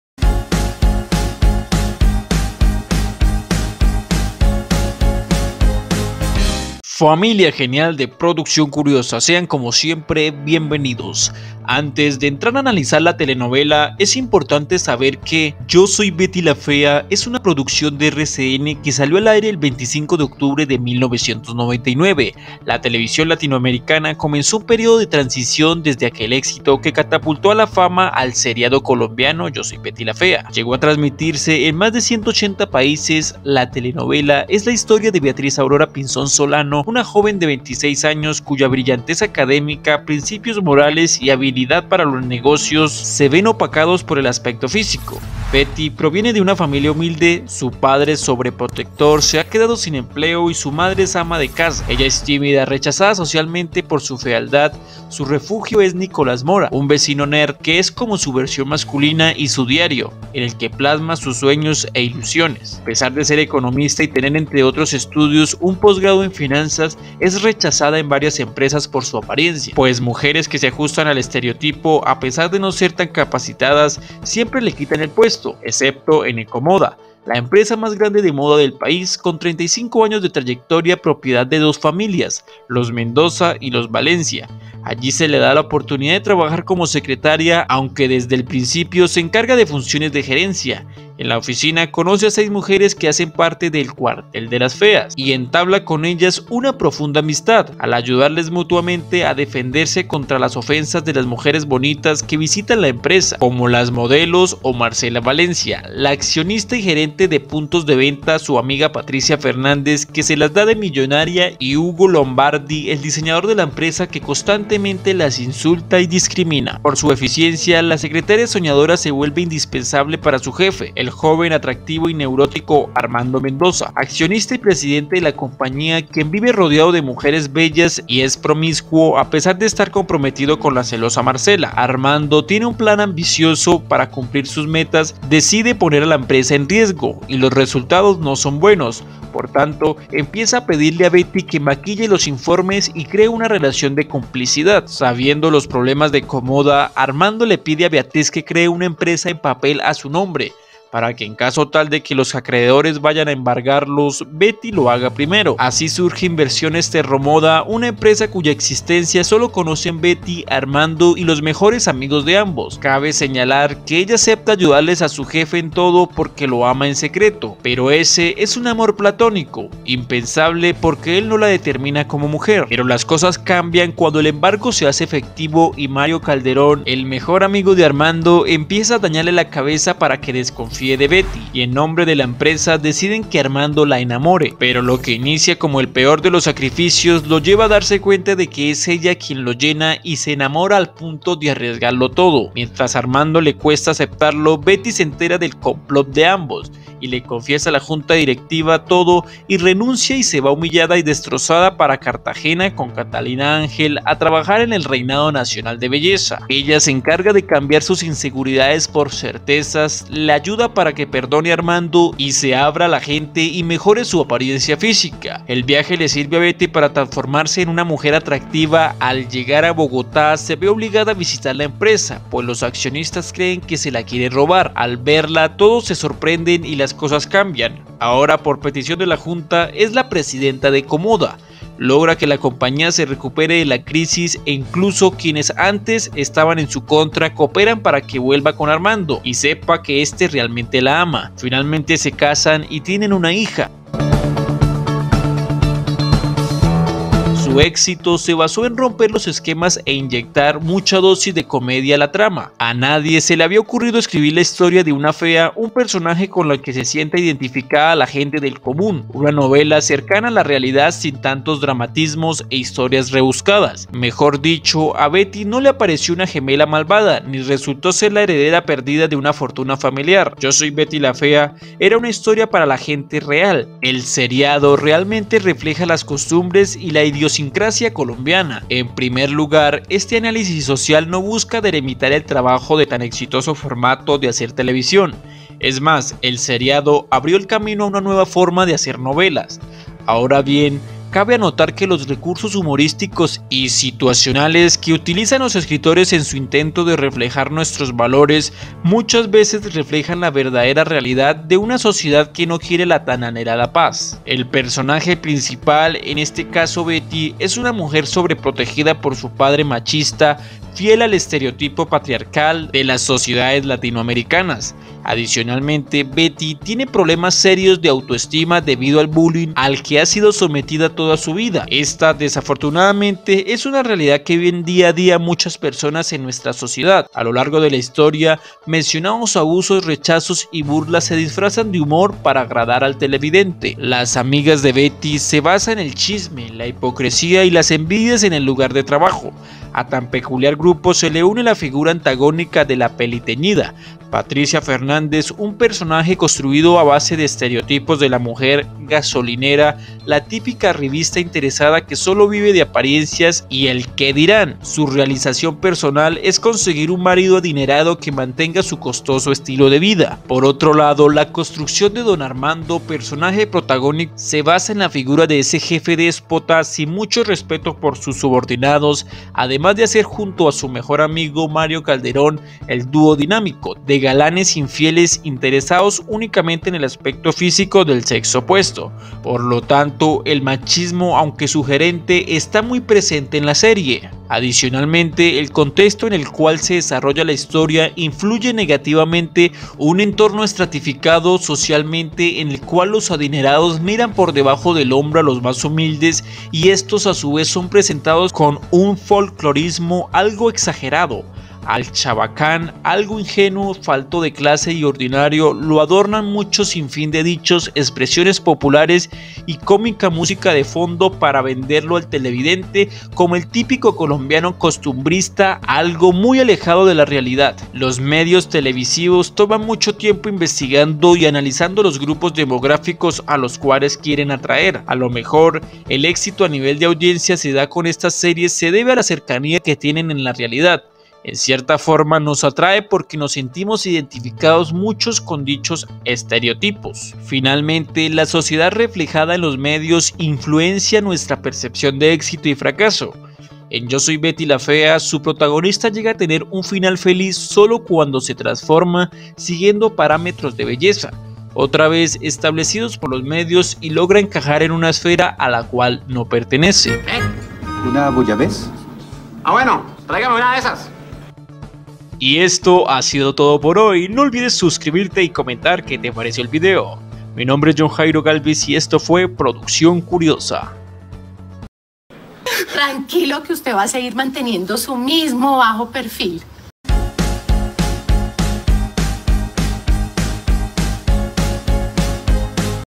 Resumen y Analisis De Betty La Fea 2020😱
Yo soy Betty, la fea o simplemente Betty, la fea es una telenovela colombiana, creada por RCN Televisión y escrita por Fernando Gaitán, ganadora del Guinness Records Se estrenó el 25 de octubre de 19991​ y finalizó el 8 de mayo de 2001.

Está protagonizada por Ana María Orozco y Jorge Enrique Abello, con las participaciones antagónicas de Natalia Ramírez, Lorna Paz, Luis Mesa y Julián Arango.

Su versión original fue emitida en muchos países con gran éxito, incluyendo a toda Iberoamérica, con elevados niveles de audiencia. Gozó de un gran éxito internacional al ser emitida en más de 180 países, ser doblada a 25 idiomas y al contar con al menos unas 28 adaptaciones alrededor del mundo.

El personaje principal es Beatriz Aurora Pinzón Solano, que es una mujer poco atractiva pero inteligente que trabaja para una compañía de modas llamada Ecomoda. Betty, como cariñosamente le dicen, tiene que soportar constantemente los insultos y desprecios de sus compañeros de trabajo, especialmente de la secretaria Patricia Fernández quien realizó seis semestres de finanzas en la San Marino, el diseñador Hugo Lombardi y la accionista de la empresa Marcela Valencia.